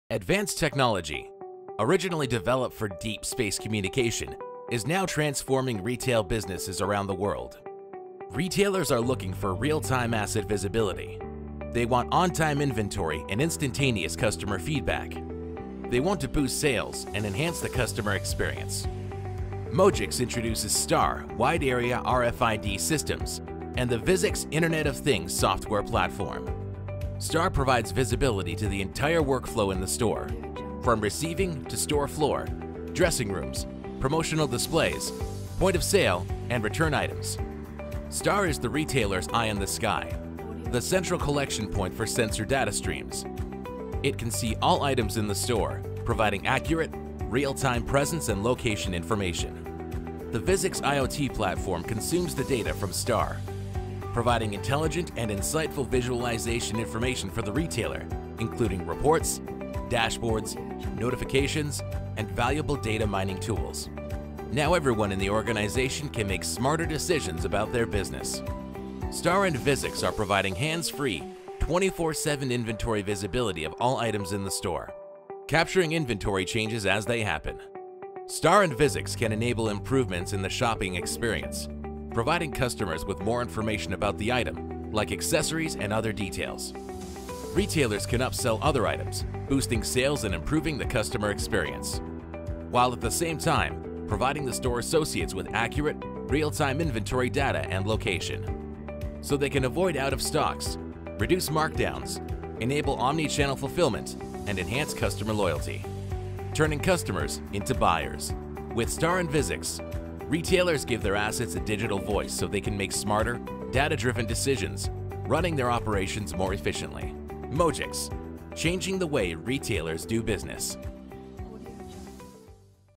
RFID Technology for Retail - Mojix
Retail technology is changing the way retailers do business. Advanced technology, originally developed for deep space communications, is now transforming retail businesses around the globe - boosting sales and enhancing the customer experience. Mojix introduces STAR wide-area RFID systems and the ViZix “internet of things” software platform.